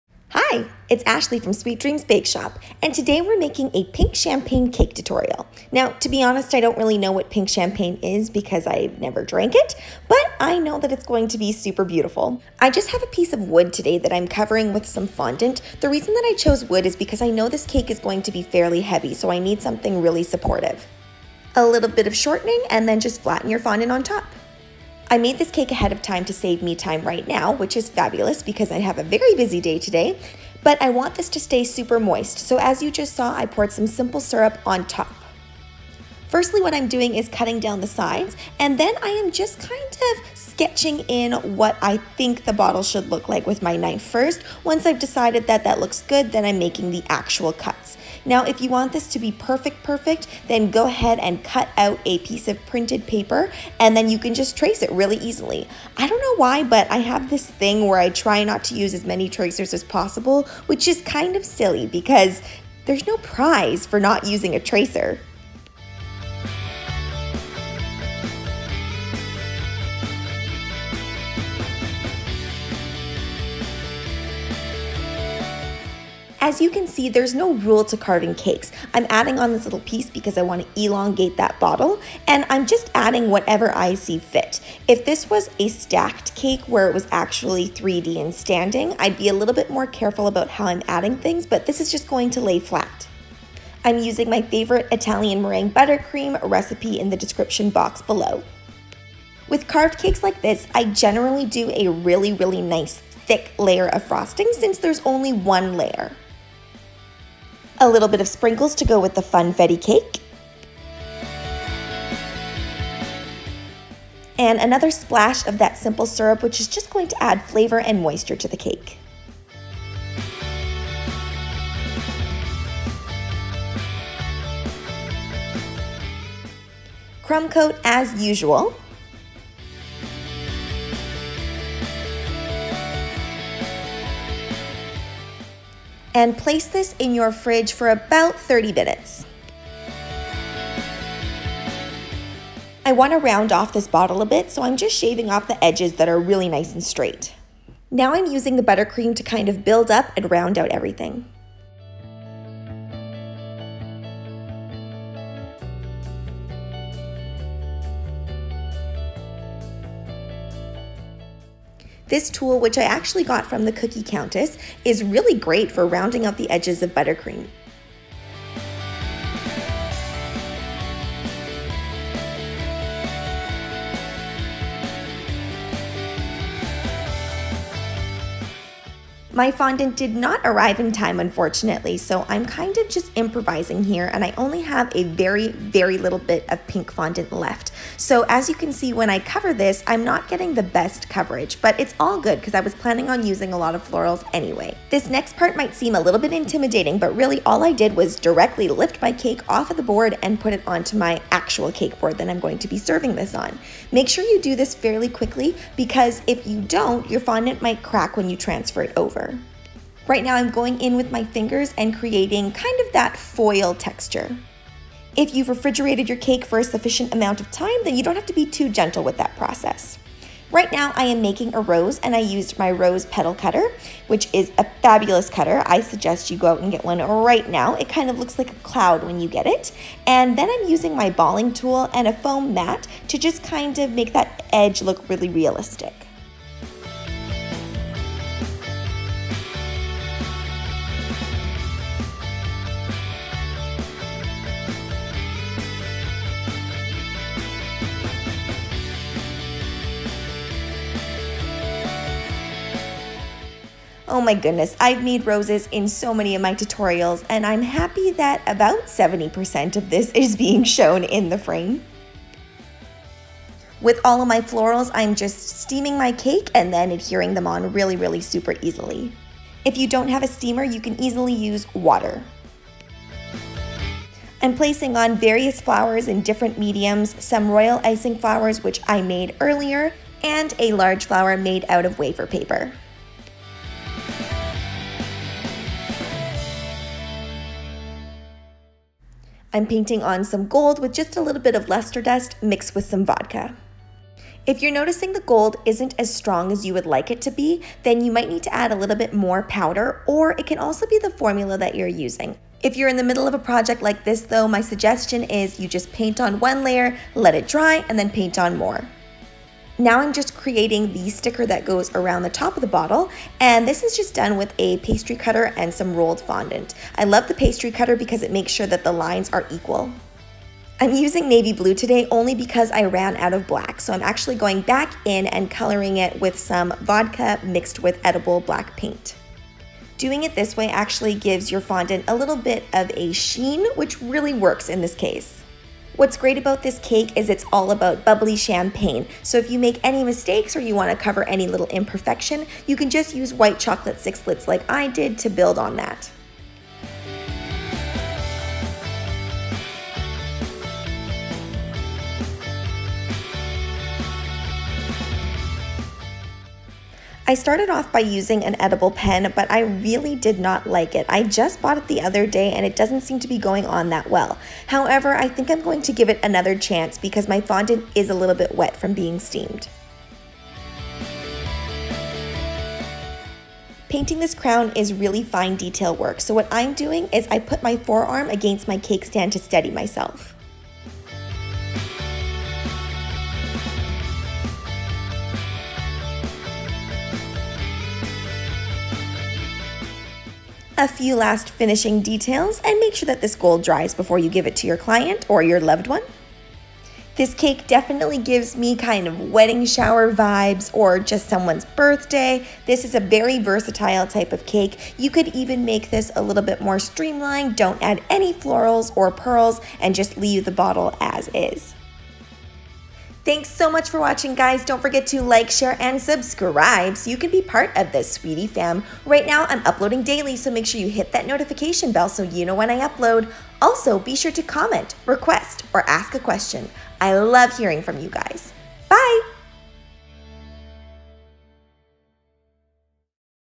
How to Make a Pink Champagne Bottle Cake | Easy Carved Fondant Cake
Today we are in celebration mode! Birthdays, wedding showers, you name it and this cake can help you celebrate it. If you're just starting out carving cakes, laying down bottle cakes are great because there's no risk of them toppling over.

Materials Used:
Satin Ice Fondant
TMP Gold Lustre Dust
Wafer Paper
Royal Icing
Chocolate Sixlets
Wooden Board
Edible Colourings

Tools Used:
Various Spatulas

Italian Meringue Buttercream Recipe:

300 grams granulated sugar
75 grams cold water
110 grams egg whites
350 grams of butter (unsalted or salted)

Method:

Combine sugar and water into a sauce pan.
Bring it up to 239 degrees F using a candy thermometer on the stove.

While you’re waiting for the sugar to come up to temp, put the egg whites into a stand mixer and begin to whip when the sugar reaches 239 degrees F; keep mixing until it gets to 244 degrees F. Remove sugar water from the stove.

Slowly pour sugar water mixture into the egg whites as they are beating.

Whip for 3 minutes on high and then add in your chopped butter.

Whip until the frosting is the consistency you desire!